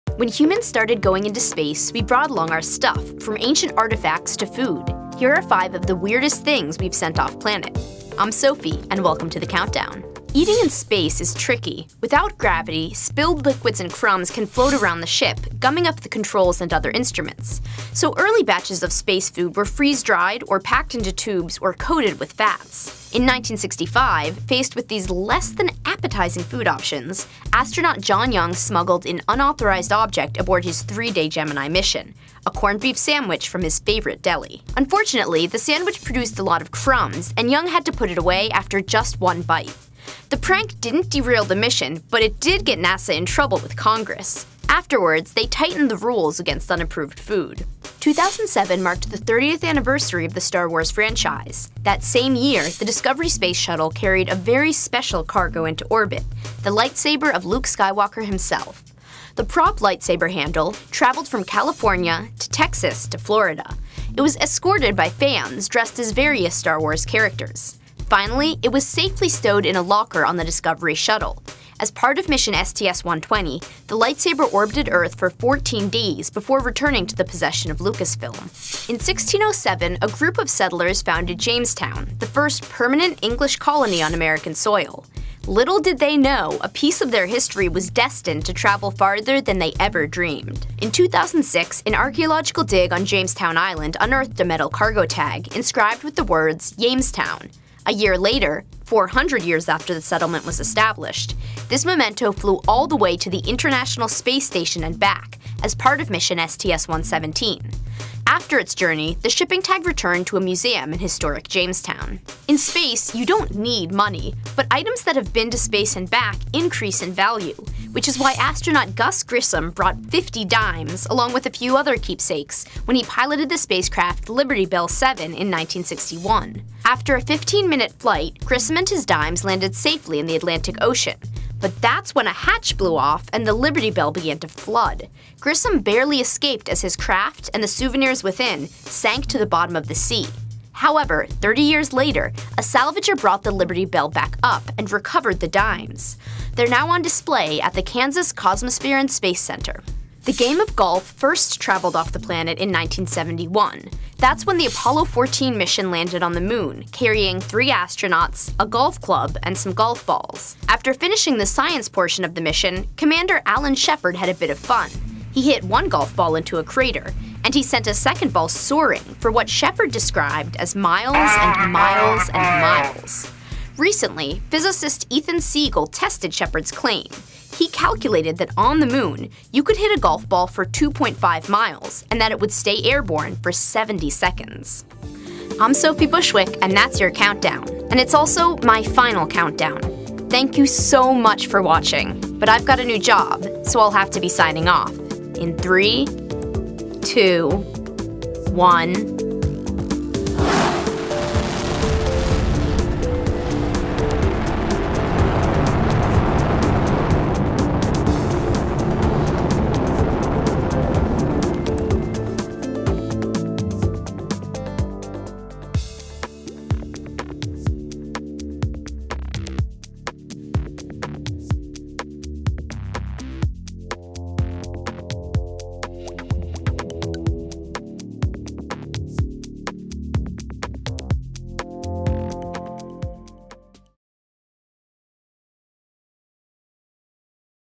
The Top 5 Strangest Things Ever Shot Into Space - The Countdown #47
Deli meat, sporting equipment and colonial artifacts are just some of the random objects astronauts have carried with them into orbit. In this episode of The Countdown we share our top five.

Host/writer: Sophie Bushwick
Production assistant: William Herkewitz
Edited by Kathryn Free & Eric R. Olson
Producer: Eric R. Olson